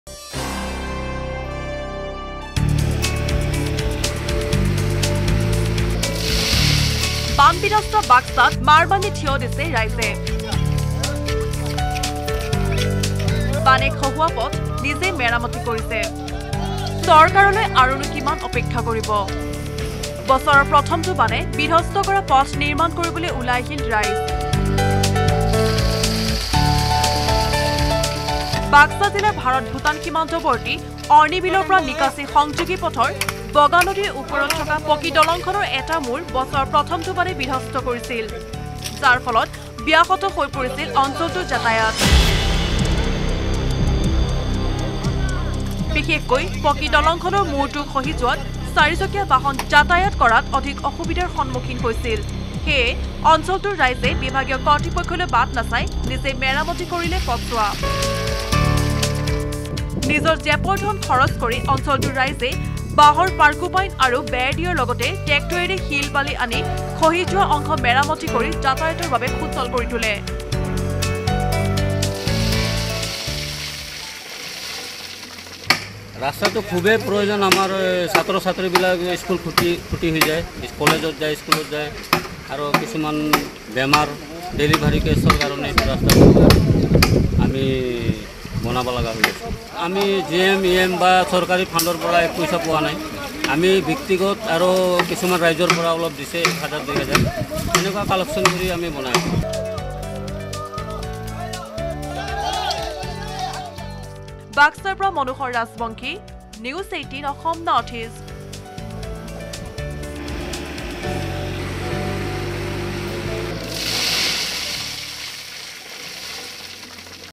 Public Repaired Road Themselves : চৰকাৰলৈ অপেক্ষা নকৰি বান ৰাইজে মেৰামতি কৰিলে পথ
Public Repaired Road Themselves | চৰকাৰলৈ অপেক্ষা নকৰি বান বিধস্ত বাক্সাত বানে খহোৱা পথ নিজেই মেৰামতি কৰি লৈছে।  জেপৰ ধন খৰচ কৰি বাহৰ পাৰ্কুপাইন আৰু বেৰ দিয়াৰ লগতে ট্ৰেক্টৰেৰে শিল বালি আনি অৰ্ণিবিলৰ পৰা নিকাছী সংযোগী পথটো মেৰামতি কৰি লৈছে।